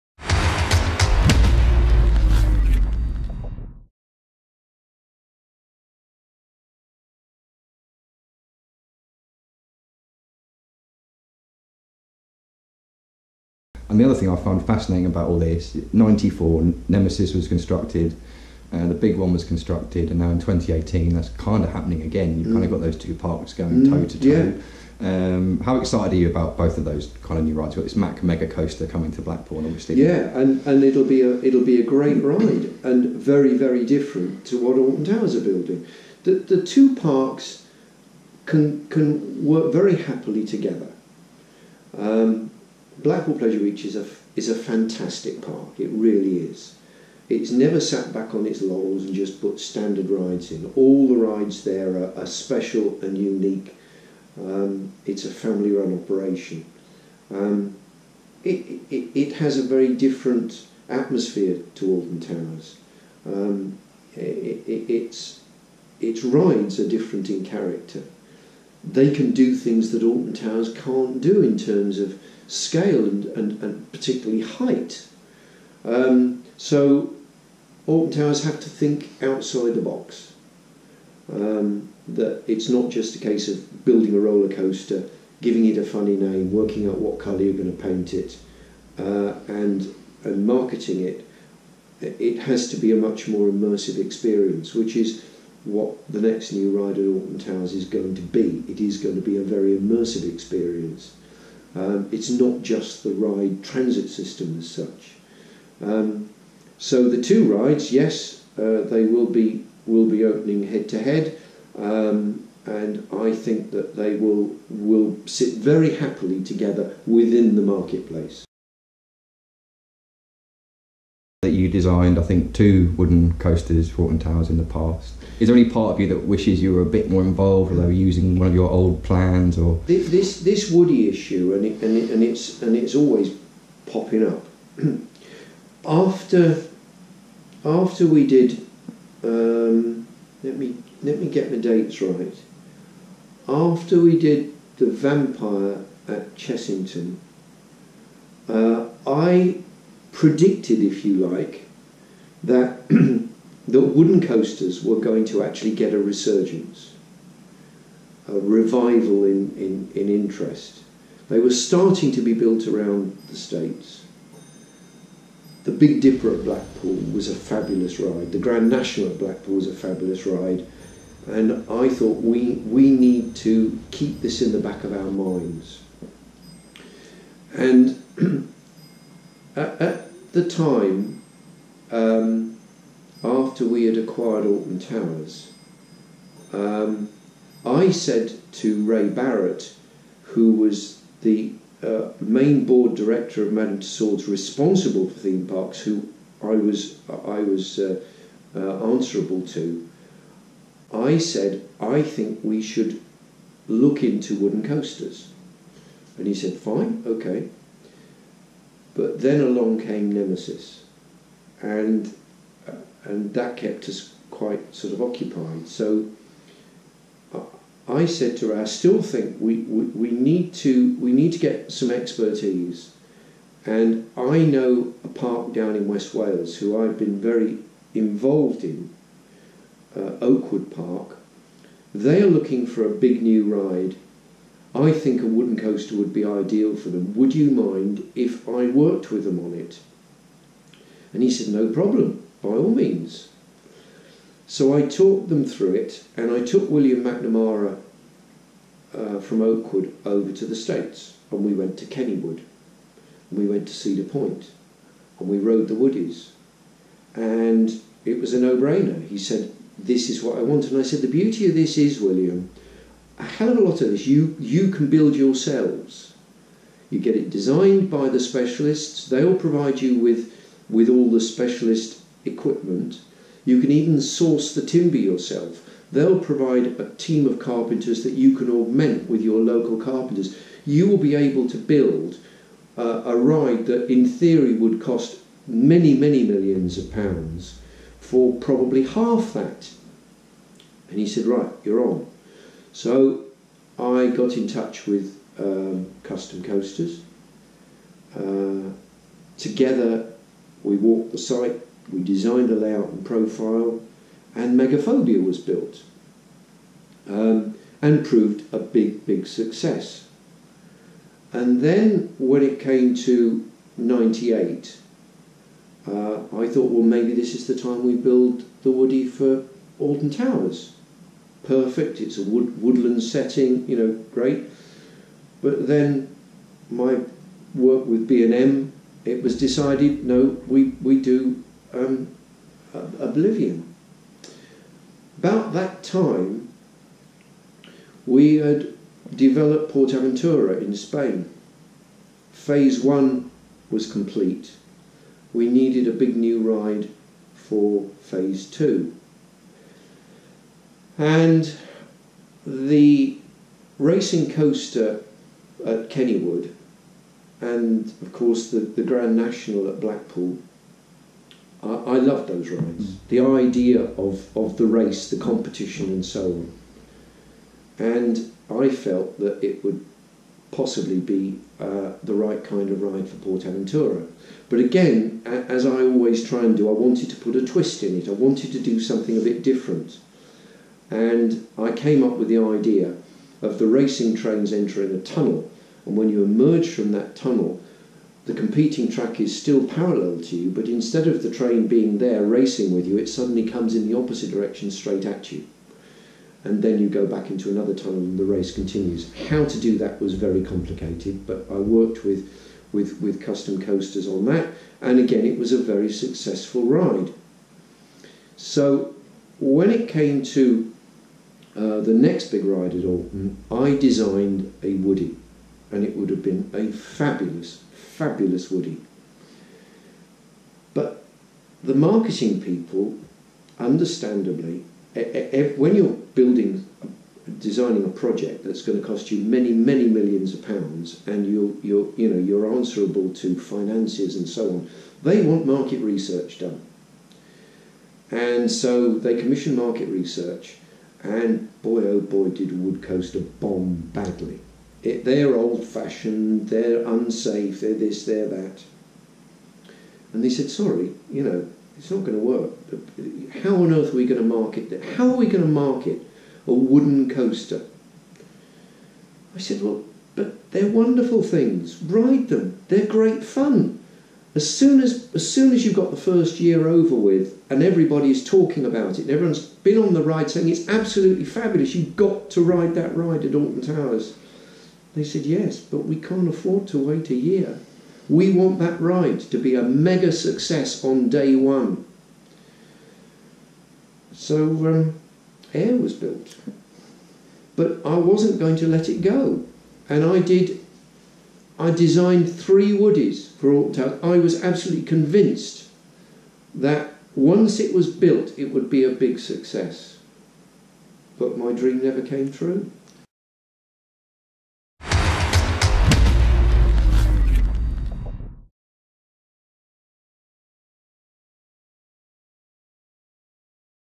John Wardley Webisodes Part 8: SW8 & ICON
In part 8 I ask John about his wooden coaster designs and we touch on two current projects, SW8 at Alton Towers and ICON at Blackpool Pleasure Beach.

With just 2 more short episodes left the series arguably peaks at
episode 8. I thought John had only designed 2 wooden coasters for Alton Towers, it turns out he designed 3!

So ironic that after his retirement Alton Towers have now gone ahead with SW8 a new wooden coaster set to open for the 2018 season.
John also talks about his work with Oakwood on Megafobia.

Additional footage from
Steve The Coaster Fan